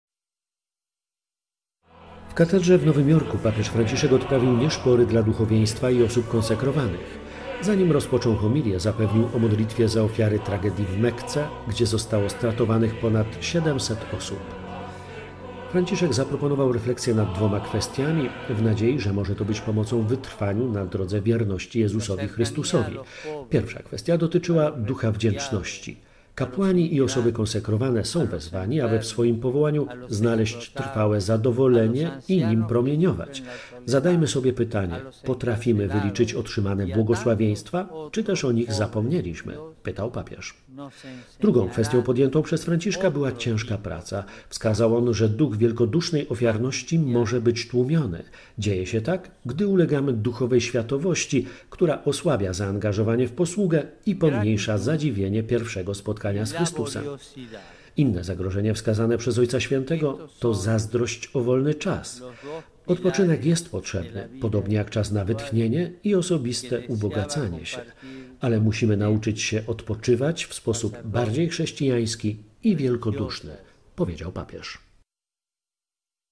Nowy Jork: Papież modlił się z duchowieństwem
W katedrze w Nowym Jorku Papież Franciszek odprawił nieszpory dla duchowieństwa i osób konsekrowanych. Zanim rozpoczął homilię, zapewnił o modlitwie za ofiary tragedii w Mekce, gdzie zostało stratowanych ponad 700 osób. W nadziei, że może to być pomocą w wytrwaniu na drodze wierności Jezusowi Chrystusowi, Franciszek zaproponował refleksję nad dwoma kwestiami. Pierwsza dotyczyła ducha wdzięczności. „Kapłani i osoby konsekrowane są wezwani, aby w swoim powołaniu znaleźć trwałe zadowolenie i nim promieniować. - Zadajmy sobie pytanie: Potrafimy wyliczyć otrzymane błogosławieństwa, czy też o nich zapomnieliśmy?” – pytał Papież.
Drugą kwestią podjętą przez Franciszka była ciężka praca. Wskazał on, że duch  ...